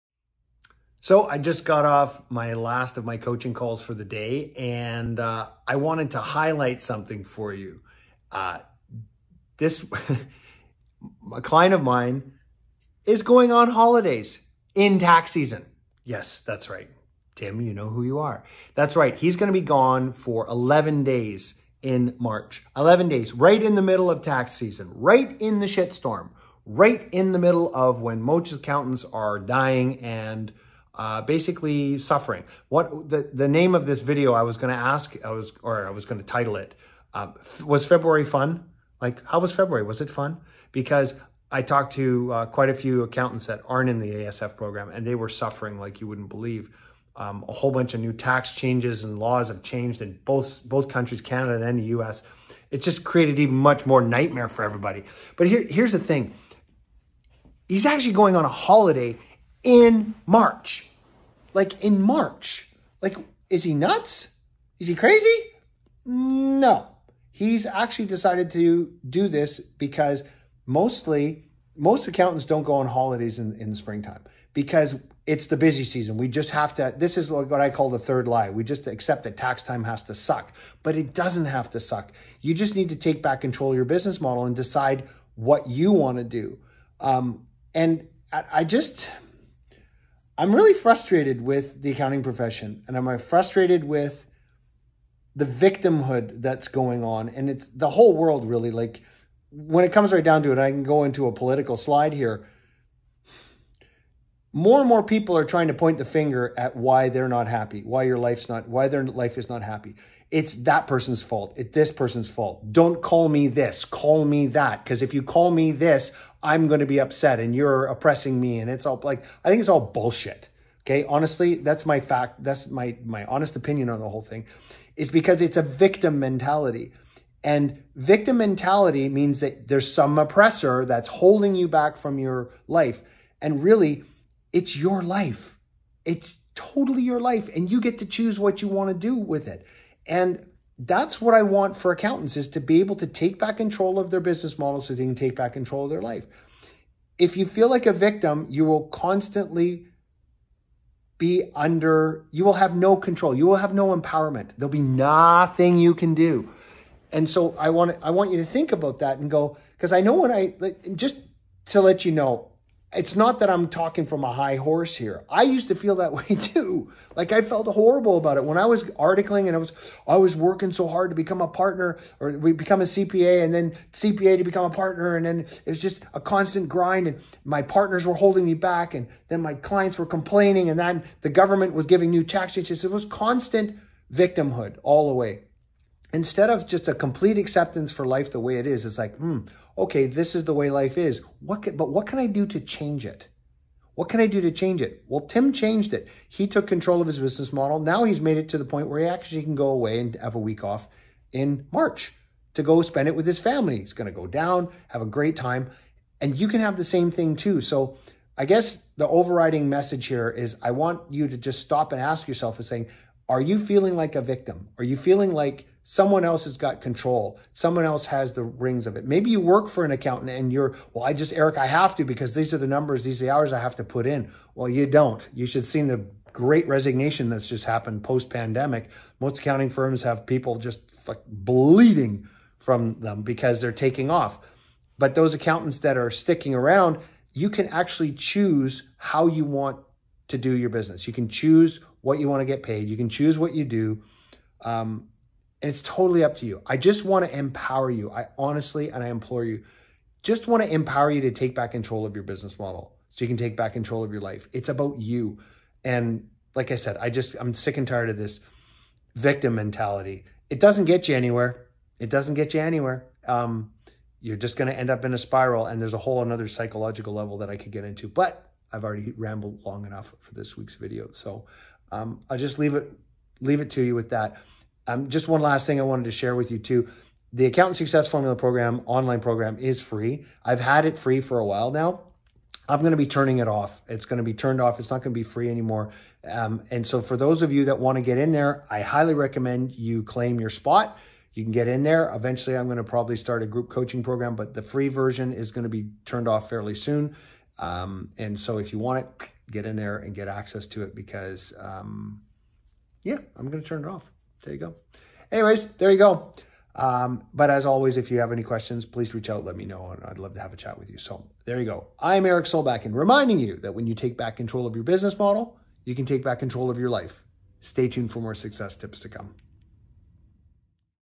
A CPA Vacationing in March?!?
Who ever heard of an accountant taking a vacation in March? It’s crazy, right?!

But it is possible.

Not only that, it’s happening. You don’t have to be chained to your desk during tax season.

This week’s video is about how one CPA did it.
…and lived to tell the tale!